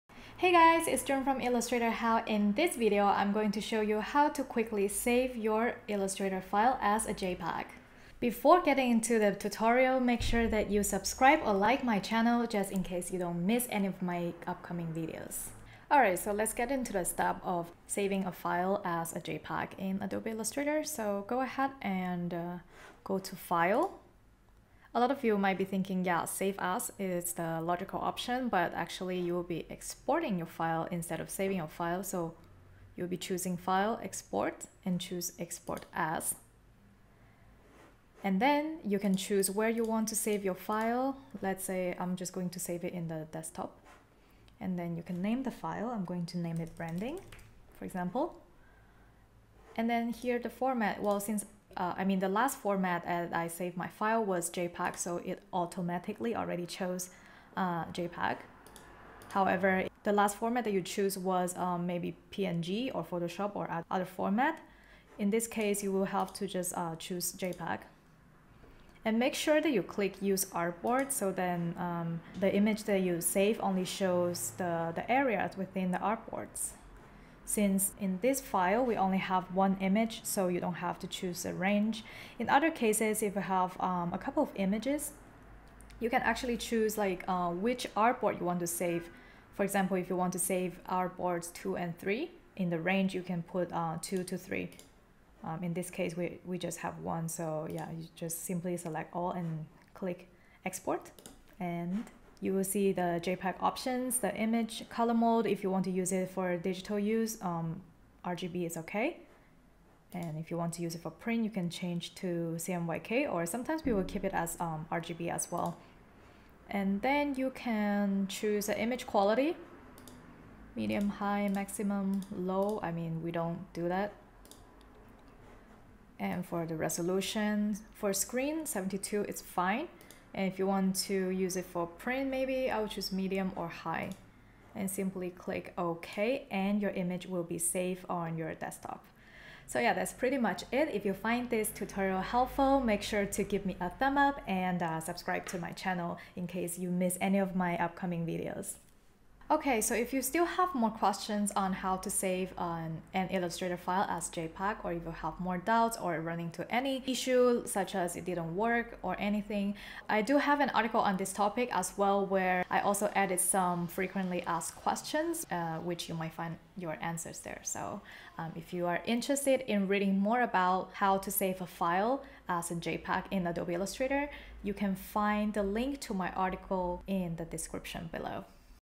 How to Save an Illustrator File as JPEG in Less than a Minute
🖼️ Learn to save your Adobe Illustrator masterpiece as a JPEG file in a few simple steps!

Can't find the JPEG format from the "Save As" option?
⚠️ The secret is, you should go to Export As!

Here are the steps:

Step 1: Go to File - Export As
Step 2: Choose JPEG as the file format
Step 3: Adjust the JPEG options

👍 Enjoyed the tutorial? Leave a comment for questions or future tutorials.